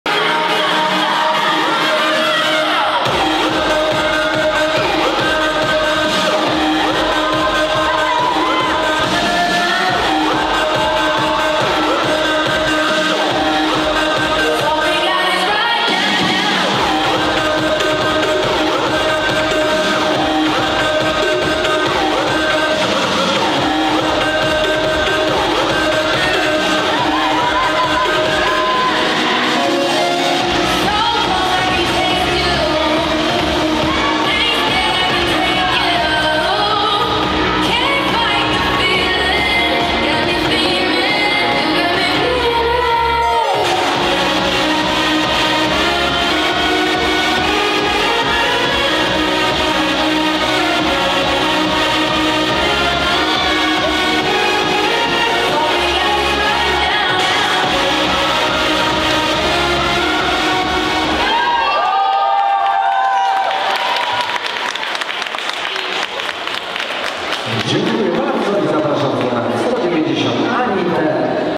MP IDO DD,DS,DDF DD Solo Female Adults 1A Aleksandra, Wiktoria
Mistrzostwa Polski PFT IDO
National Disco Champs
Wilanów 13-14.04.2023

Disco Dance  Solo Female  Adults 1A
Aleksandra Bodych,
Wiktoria Baraniak